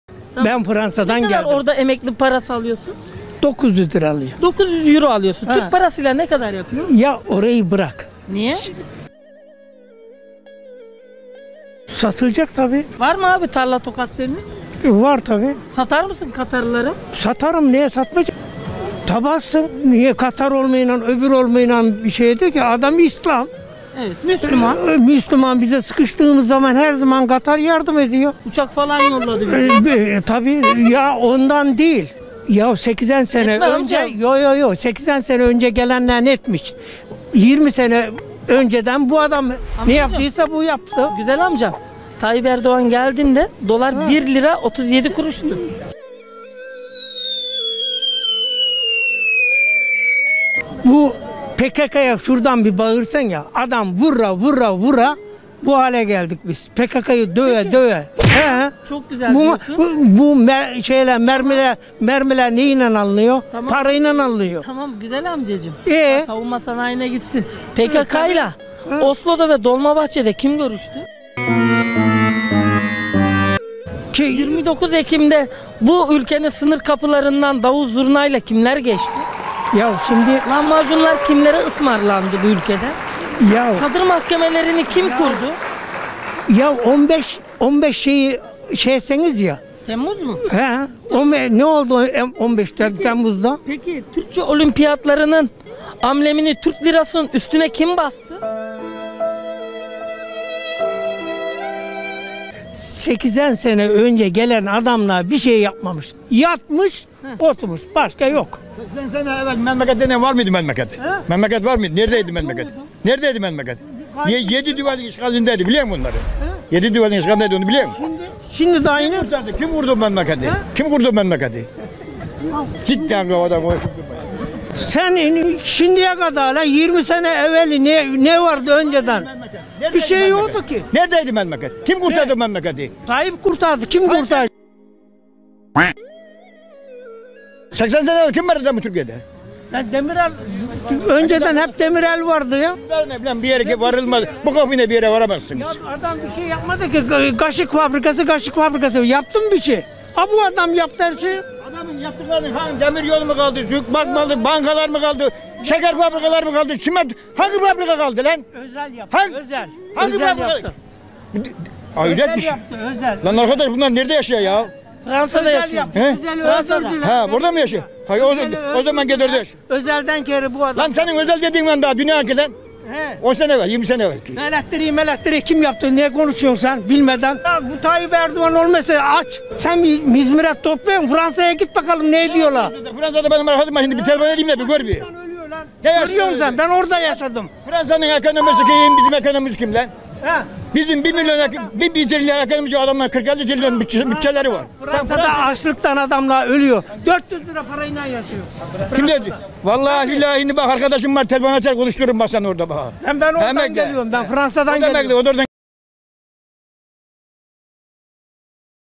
FRANSADA YAŞAYAN GURBETÇİ BEYİNLERİ YAKAN RÖPORTAJ VERDİ.EDİT VERSİYON KOMEDİ SOKAK RÖPORTAJI
GURBETÇİ AMCA TÜRKİYEDE EKONOMİ ÇOK GÜZEL DİYOR.20 SENE ÖNCE YOL YOKTU DİYOR,SU YOKTU DİYOR.KOMEDİ EDİT VERSİYON.DEEP HUMOR.
video @ilavetv kanalından alınmıştır.